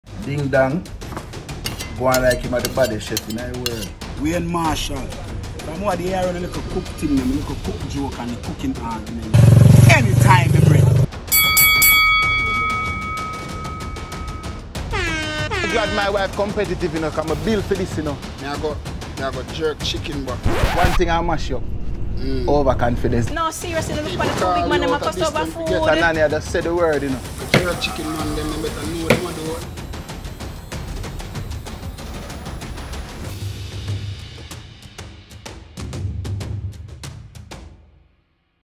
ChillinWidGrace Teaser - Chef It Wayne Marshall vs Ding Dong Cookoff
We're sure by now, you all saw the infamous challenge 👀
Chef It Wayne from @MeetTheMitchells has trained and seh a time fi challenge a veteran!
Well, he made the challenge and Chef Ding answered! Set yuh reminders cause on Saturday, July 31st, we a go see who a baddest inna the kitchen!

Comment and let us know, are you TeamWayne or TeamDing ?